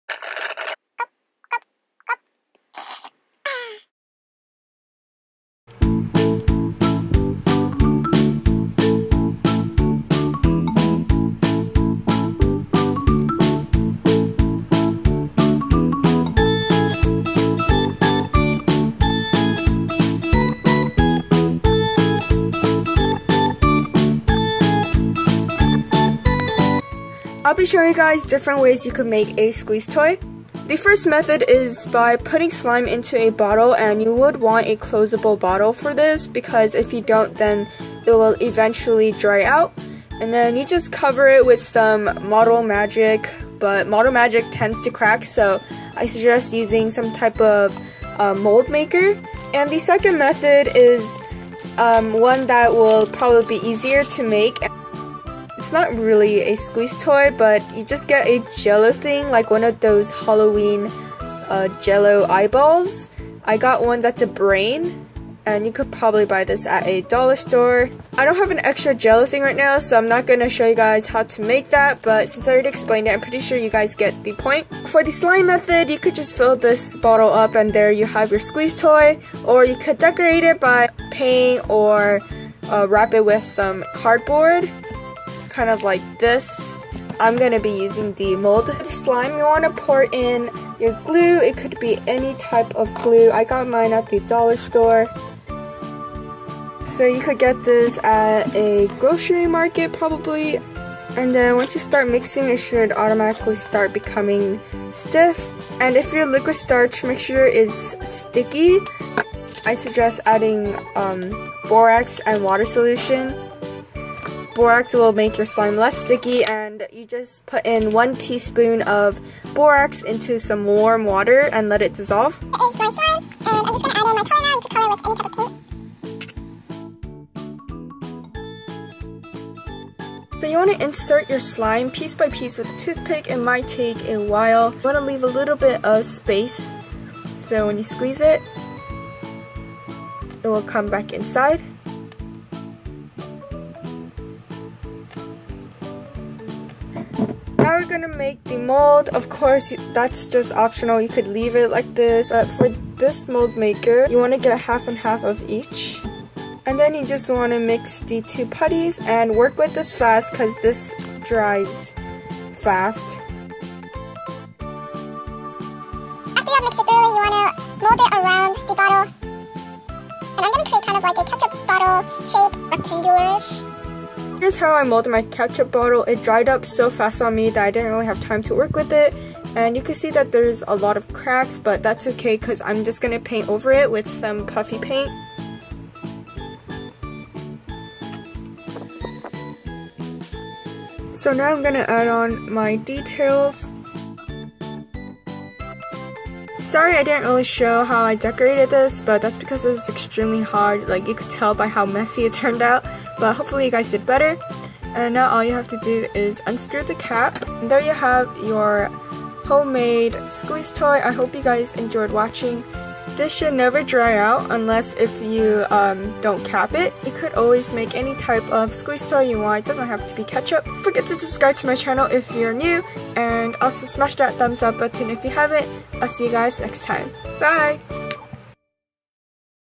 DIY Ketchup Squeeze Toy
For the slime mix glue with any type of slime activator such as borax, liquid starch, eyedrops, baking soda,etc.